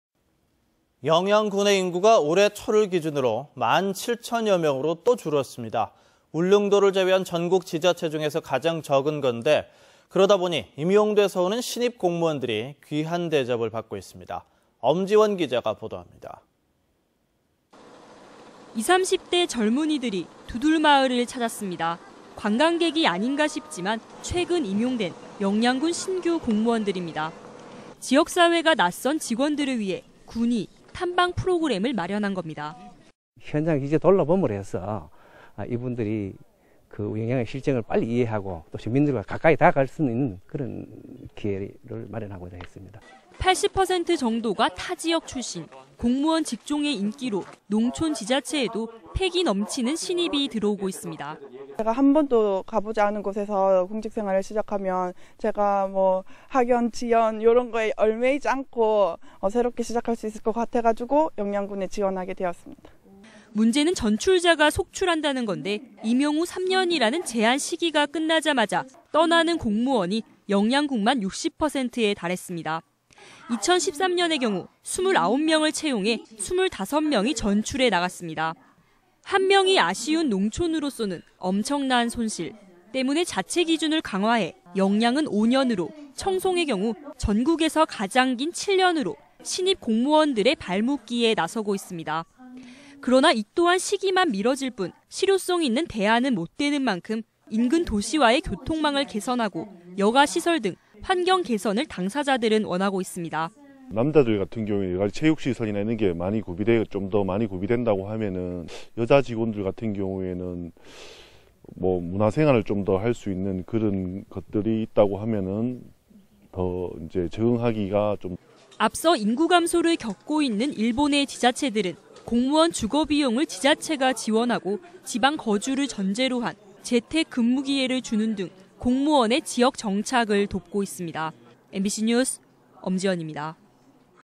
[대구MBC뉴스] 농촌 지자체, 공무원 붙잡기 안간힘
방송: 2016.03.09 (수) 대구MBC 뉴스투데이
내용: 농촌 지자체, 공무원 붙잡기 안간힘 - 엄지원 기자 (안동M)